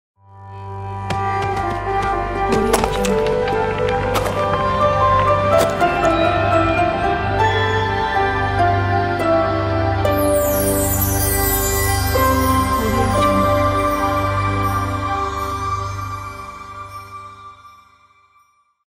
Black Stone Logo Reveal (Top After Effects Templates)
It will be a great inspiration for us to create more useful projects for Customers like you.

Hi

The Black Stone Logo Reveal is very easy to edit just replace the logo or any Text and ready to render.It is useful to reveal the logo and text of your company.

Compatible with After Effects CS5, CS5.5, CS6, CC, CC 2014 or CC 2015

3D Stone is prerendered

“Video tutorial”

24 fps

Audio created by AmethistLab

Music used in a preview is specially composed for this project by AmethistLab , and is not included with the project files, but you can get it from this link:

Thanks

Abhishek